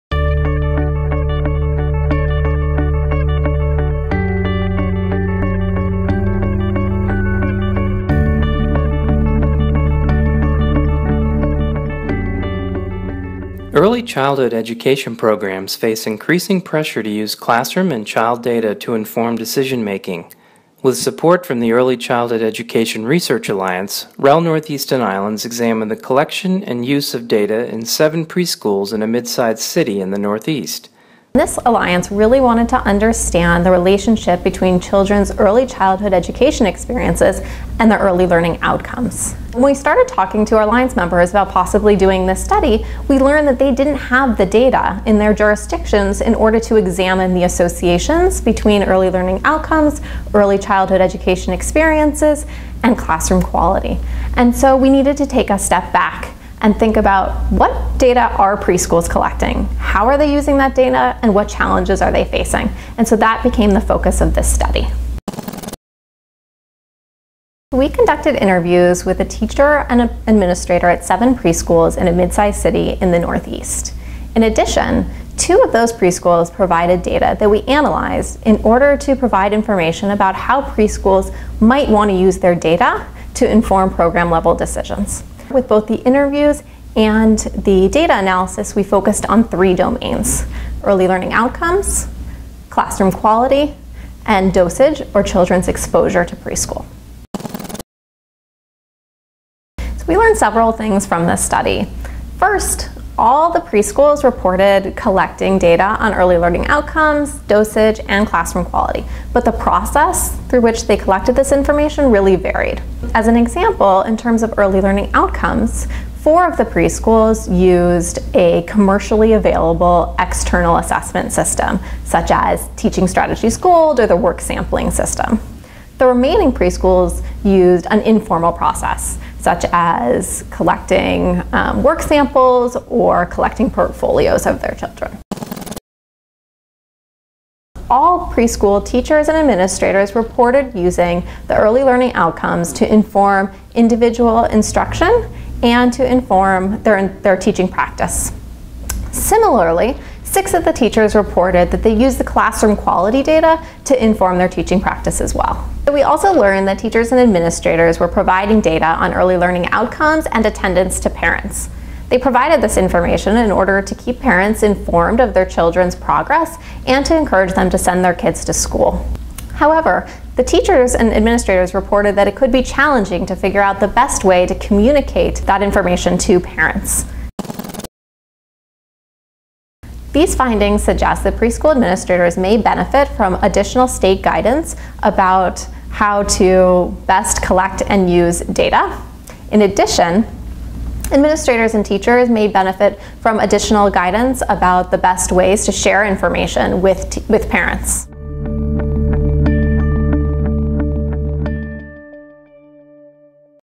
Study Findings: Data Collection and Use in Early Childhood Programs, Evidence from the Northeast
Early childhood education programs face increasing pressures to collect data, about both teachers and children, and to use those data to make decisions. Research supports the potential value of using data in education settings for multiple purposes, but little is known about whether or how early childhood education programs use data for these purposes. A REL Northeast & Islands study explored how early childhood education programs are collecting and using data, how they would like to use data, how they could use the data that they have, and the challenges they face in these efforts. These tasks were accomplished by interviewing administrators and teachers at seven preschools in a mid-sized city in the Northeast region and by analyzing child data already collected by two of these preschools. This study was conducted in collaboration with the Early Childhood Education Research Alliance at REL Northeast & Islands. To read the full study and Stated Briefly report,

The content of this video does not necessarily reflect the views or policies of the Institute of Education Sciences or the U.S. Department of Education, nor does mention of trade names, commercial products, or organizations imply endorsement by the U.S. Government.